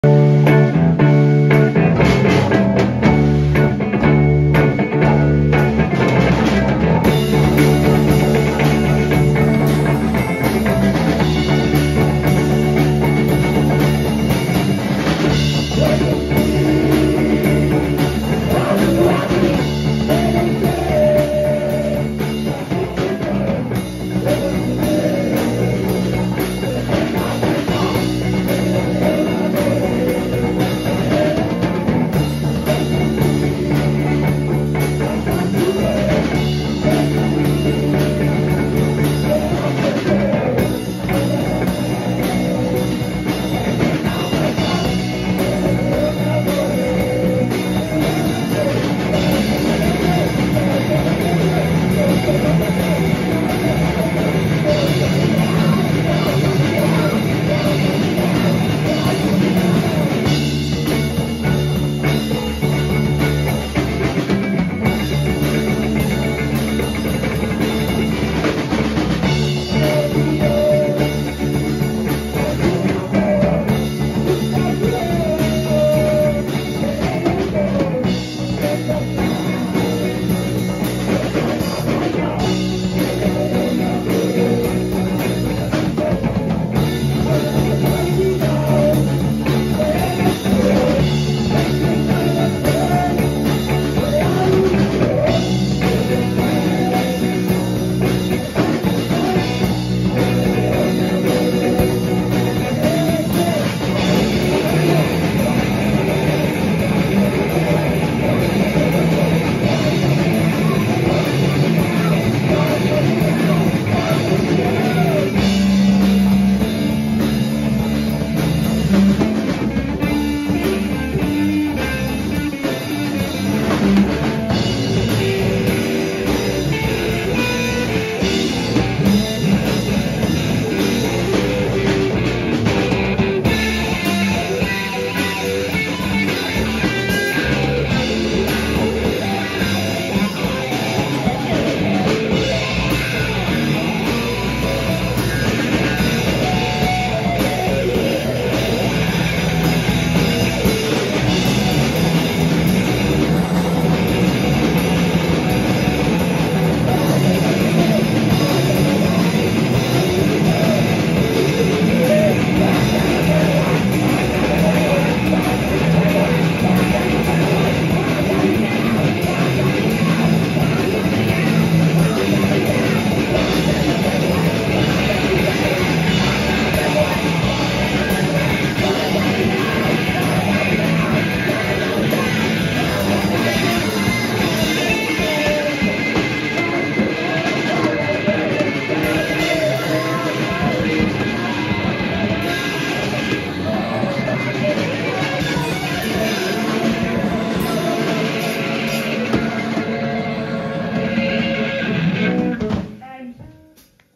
Black Wesker Live at The Duke Neath 2023
Footage captured by the dude from The Rock Foxes (check them out after you've listened to Black Wesker!)